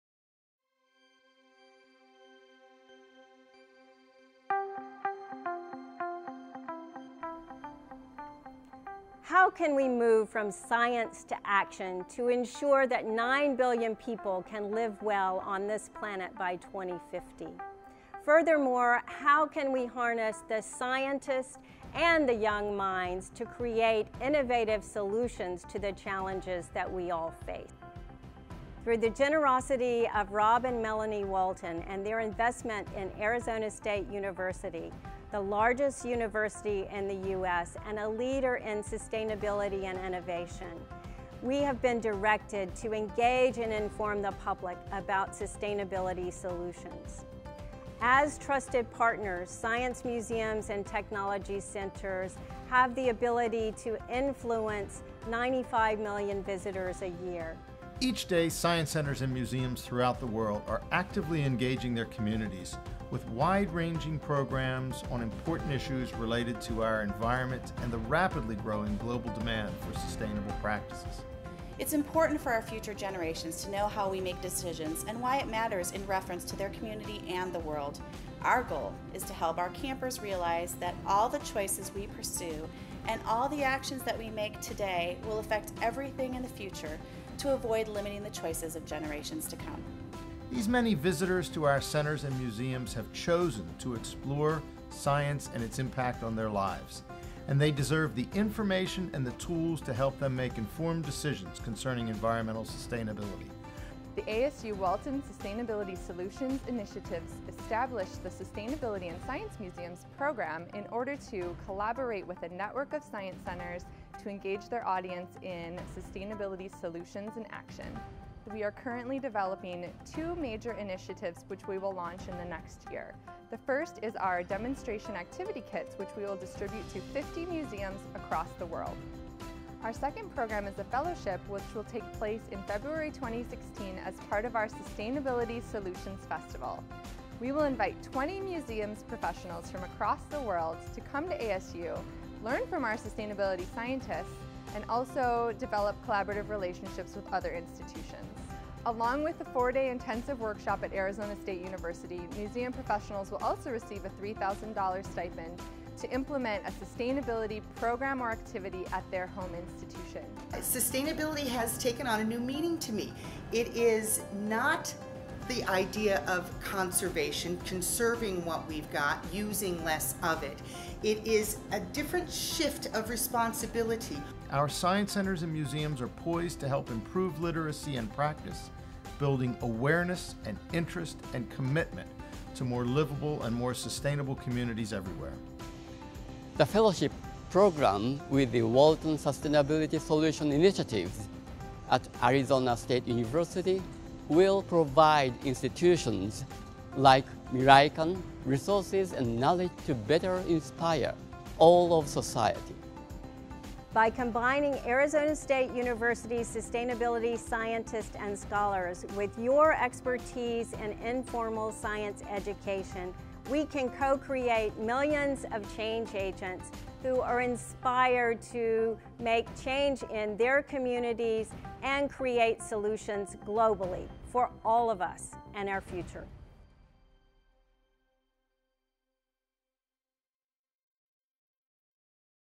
Sustainability in Science Museums
The Walton Sustainability Solutions Initiatives at Arizona State University invite science museums professionals from across the globe to engage in the programs and activities of the Sustainability in Science Museums initiative.